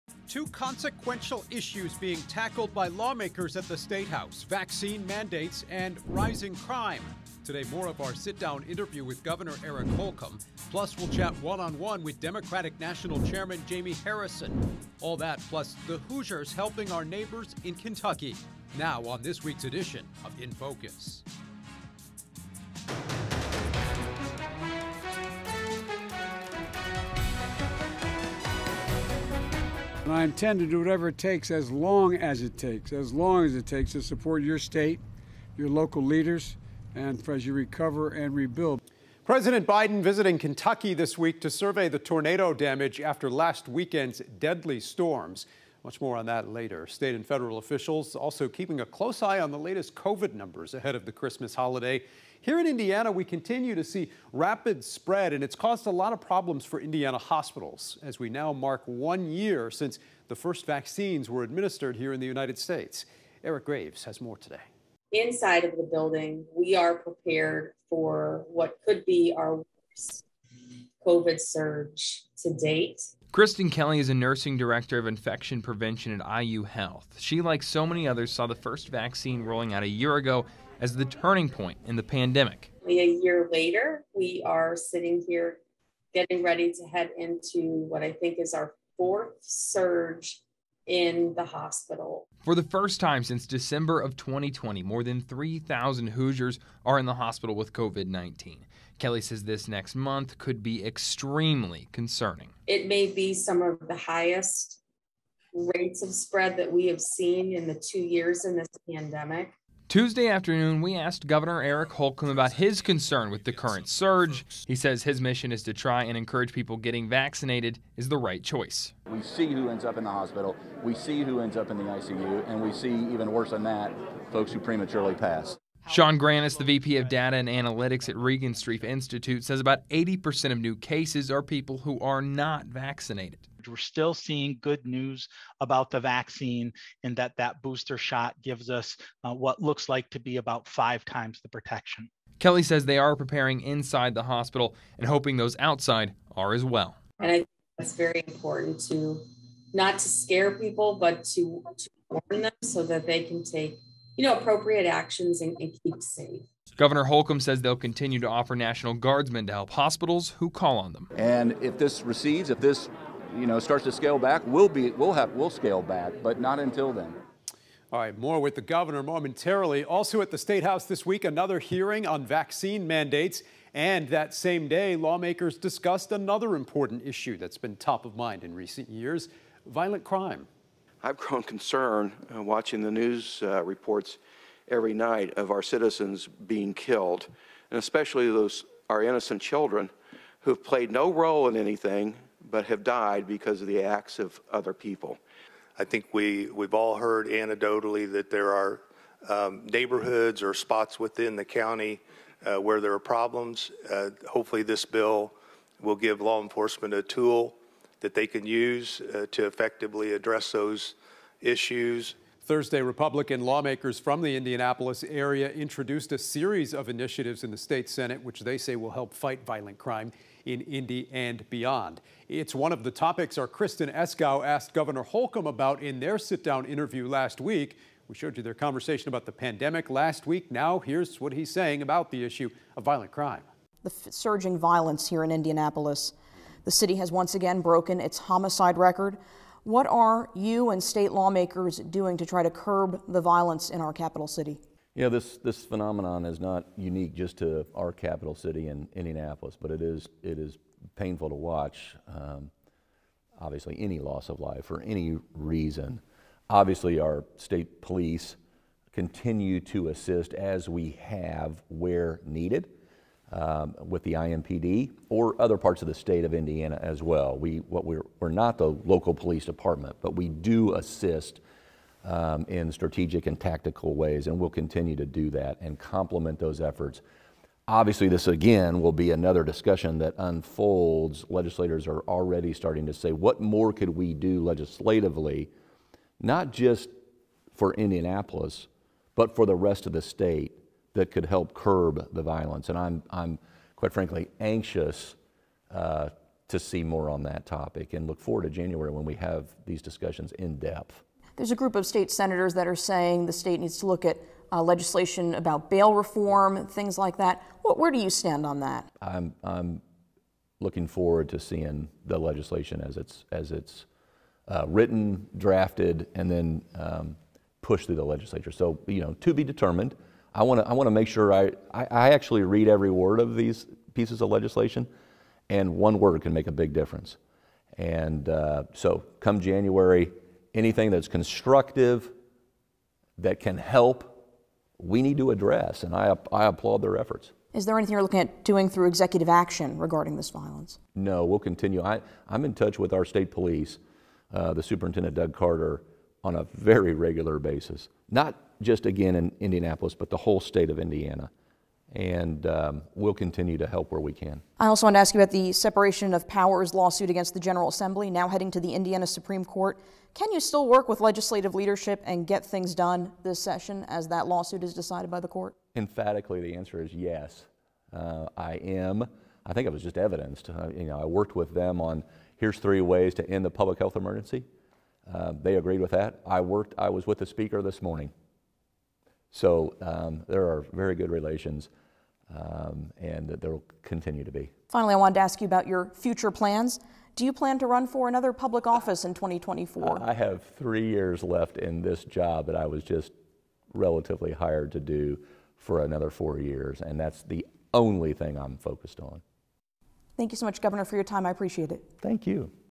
IN Focus: Gov. Eric Holcomb, state lawmakers on reducing Indiana crime
Amid a record-breaking year for homicides in Indianapolis, and rising crime throughout Indiana, state lawmakers say they are preparing new measures to fight violence in the city.